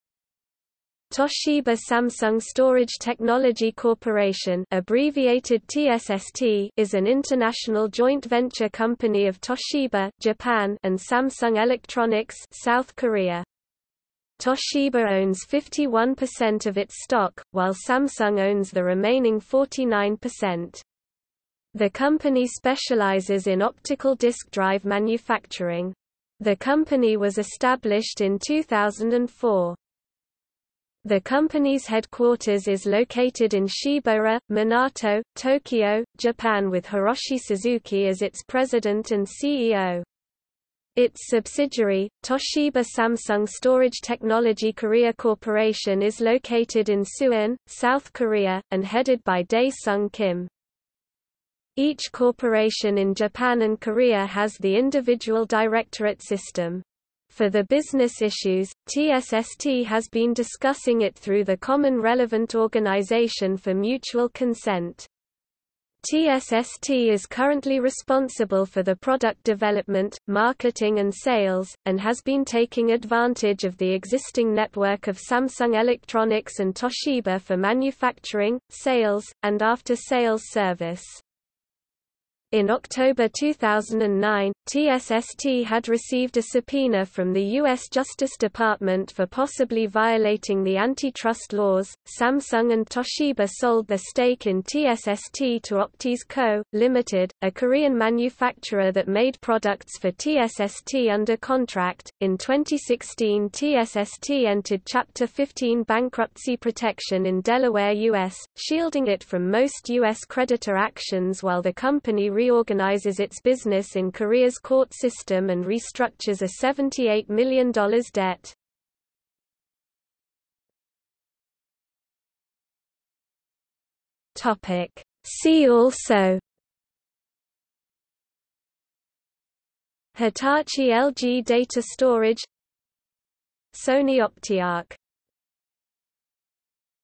Toshiba Samsung Storage Technology | Wikipedia audio article
00:02:00 See also

- Socrates

SUMMARY
Toshiba Samsung Storage Technology Corporation (abbreviated TSST) is an international joint venture company of Toshiba (Japan) and Samsung Electronics (South Korea).  Toshiba owns 51% of its stock, while Samsung owns the remaining 49%.  The company specialises in optical disc drive manufacturing.  The company was established in 2004.

The company's headquarters is located in Shibaura, Minato, Tokyo, Japan with Hiroshi Suzuki as its President and CEO. Its subsidiary, Toshiba Samsung Storage Technology Korea Corporation is located in Suwon, South Korea, and headed by Dae Sung Kim.
Each corporation in Japan and Korea has the individual directorate system.  For the business issues, TSST has been discussing it through the common relevant organization for mutual consent.  TSST is currently responsible for the product development, marketing and sales, and has been taking advantage of the existing network of Samsung Electronics and Toshiba for manufacturing, sales, and after-sales service.
In October 2009, TSST had received a subpoena from the U.S. Justice Department for possibly violating the antitrust laws.Samsung and Toshiba sold their stake in TSST to Optis Co., Ltd., a Korean manufacturer that made products for TSST under contract.In 2016 TSST entered Chapter 15 bankruptcy protection in Delaware US, shielding it from most U.S. creditor actions while the company reorganizes its business in Korea’s court system and restructures a $78 million debt.